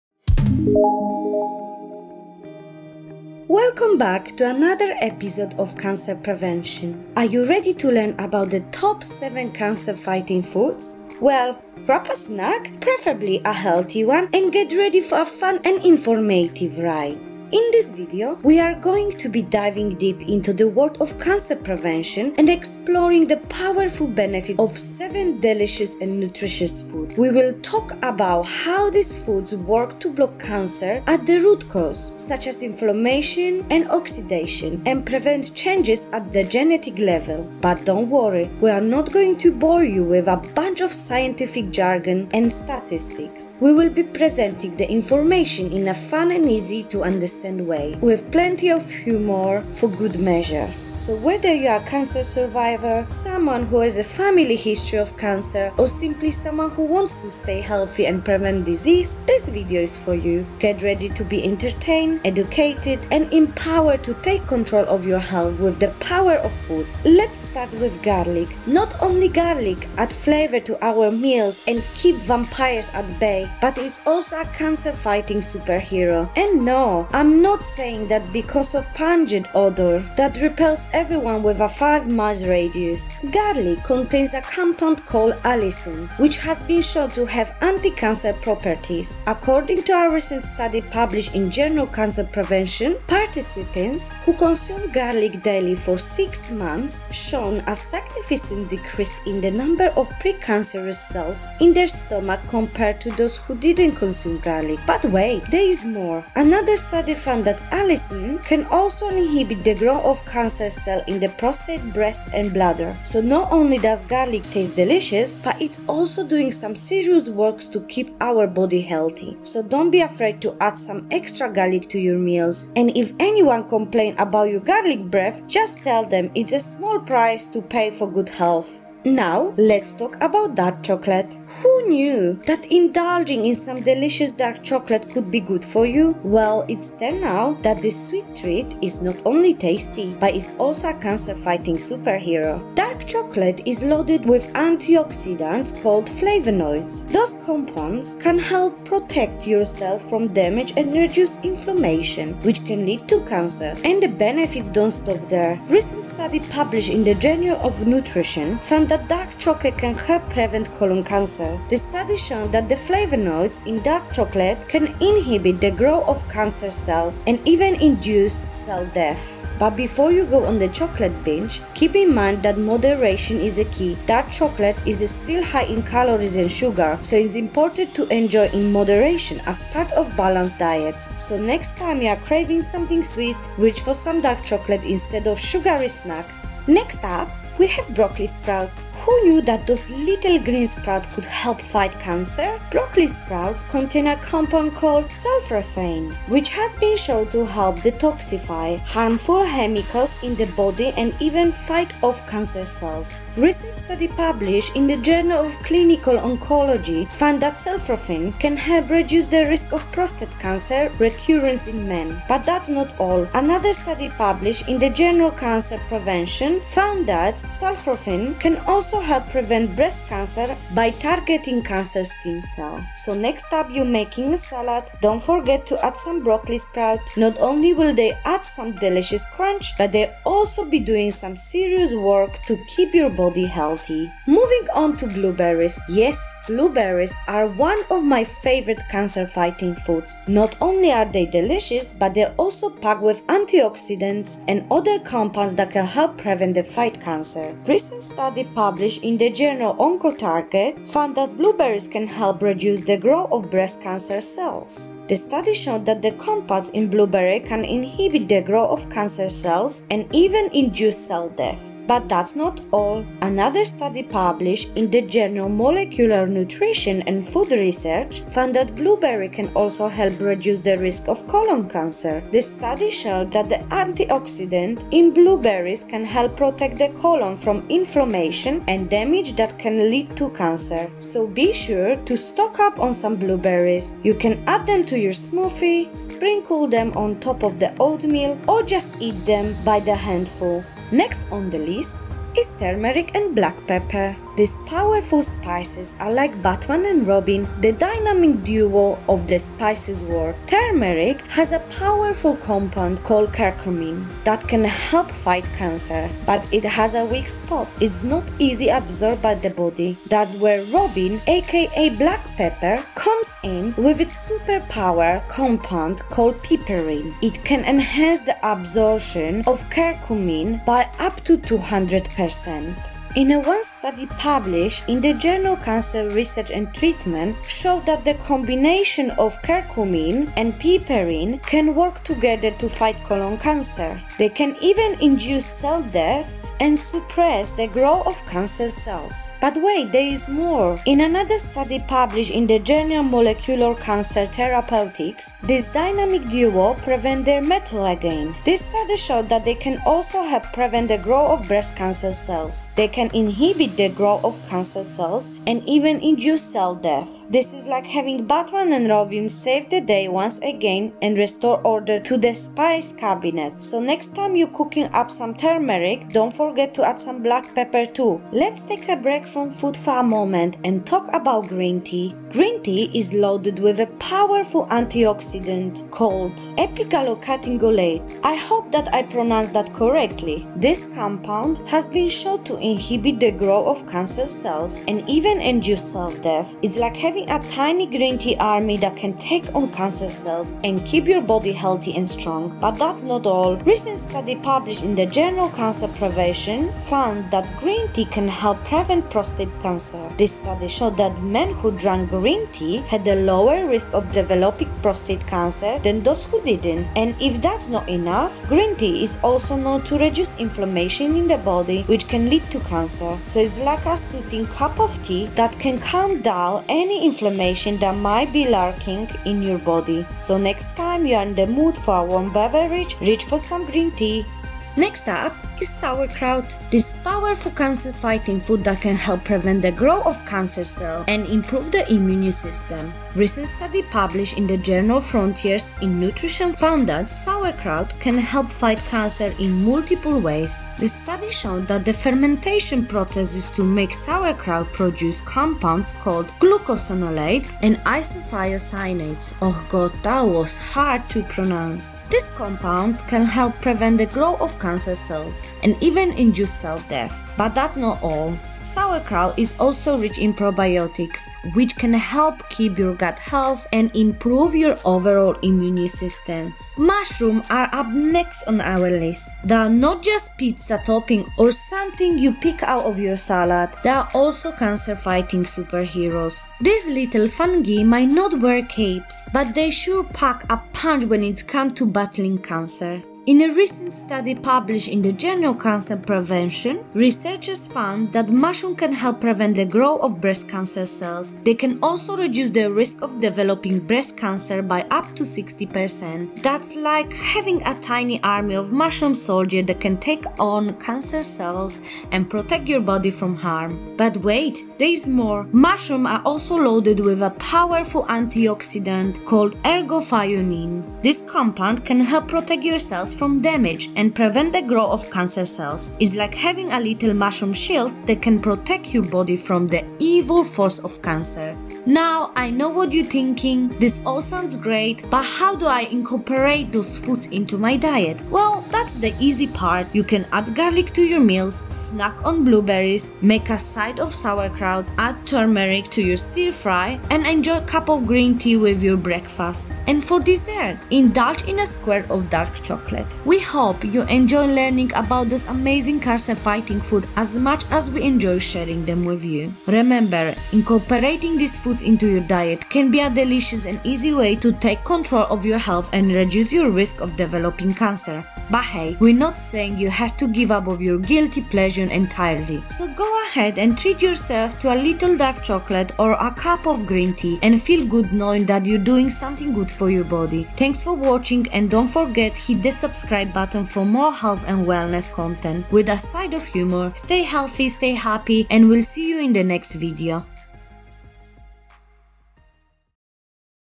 Top 8 Cancer-Fighting Foods. Science Reveals All!!!
You Won't Believe What These 8 Foods Can Do to Cancer! Top 8 Cancer-Fighting Foods. Science Reveals All!!!

In this video, we're going to be diving deep into the world of cancer prevention and exploring the powerful benefits of eight delicious and nutritious foods. We'll talk about how these foods work to block cancer at the root cause, such as inflammation and oxidation, and prevent changes at the genetic level.

So, whether you're a cancer survivor, someone who has a family history of cancer, or simply someone who wants to stay healthy and prevent disease, this video is for you! Get ready to be entertained, educated, and empowered to take control of your health with the power of food.

These 8 Foods? If Not, You're Missing Out on Cancer-Fighting Benefits!

Garlic contains a compound called allicin, which has been shown to have anti-cancer properties. According to a recent study published in the journal Cancer Prevention Research, participants who consumed garlic daily for 6 months showed a significant decrease in the number of pre-cancerous cells in their stomachs compared to those who didn't consume garlic.

Dark chocolate is loaded with antioxidants called flavonoids. These compounds can help protect your cells from damage and reduce inflammation, which can lead to cancer. A recent study published in the Journal of Nutrition found that dark chocolate can help prevent colon cancer. The study showed that the flavonoids in dark chocolate can inhibit the growth of cancer cells and even induce cell death.

Broccoli sprouts contain a compound called sulforaphane, which has been shown to help detoxify harmful chemicals in the body and even fight off cancer cells.  A recent study published in the Journal of Clinical Oncology found that sulforaphane can help reduce the risk of prostate cancer recurrence in men.

Blueberries are one of my favorite cancer-fighting foods. Not only are they delicious, but they're also packed with antioxidants and other compounds that can help prevent and fight cancer. A recent study published in the journal Oncotarget found that blueberries can help reduce the growth of breast cancer cells. The study showed that the compounds in blueberries can inhibit the growth of cancer cells and even induce cell death.

Turmeric & Black Pepper. These powerful spices are like Batman and Robin, the dynamic duo of the spice world. Turmeric has a powerful compound called curcumin that can help fight cancer. But, it has a weak spot – it's not easily absorbed by the body. That's where Robin, aka black pepper, comes in with its superpower compound called piperine A recent study published in the journal Cancer Research and Treatment showed that the combination of curcumin and piperine can work together to fight colon cancer cells. They can even induce cell death and suppress the growth of cancer cells.

Green tea is loaded with a powerful antioxidant called EGCG (epigallocatechin gallate). This compound has been shown to inhibit the growth of cancer cells and even induce cell death
But that's not all! A recent study published in the journal Cancer Prevention Research found that green tea can help prevent prostate cancer. The study showed that men who drank green tea had a lower risk of developing prostate cancer than those who didn't. And if that's not enough, green tea is also known to reduce inflammation in the body, which can lead to cancer.

Sauerkraut The powerful cancer-fighting food can help prevent the growth of cancer cells and improve your immune system. A recent study published in the journal Frontiers in Nutrition found that sauerkraut can help fight cancer in multiple ways. The study showed that the fermentation process used to make sauerkraut produces compounds called glucosinolates and isothiocyanates. These compounds can help prevent the growth of cancer cells and even induce cell death.

Mushrooms They're not just a pizza topping or something you pick out of your salad, they're also cancer-fighting superheroes. In a recent study published in the journal Cancer Prevention Research, researchers found that mushrooms can help prevent the growth of breast cancer cells. They can also reduce the risk of developing breast cancer by up to 60%!
Mushrooms are also loaded with a powerful antioxidant called ergothioneine. This compound can help protect your cells from damage and prevent the growth of cancer cells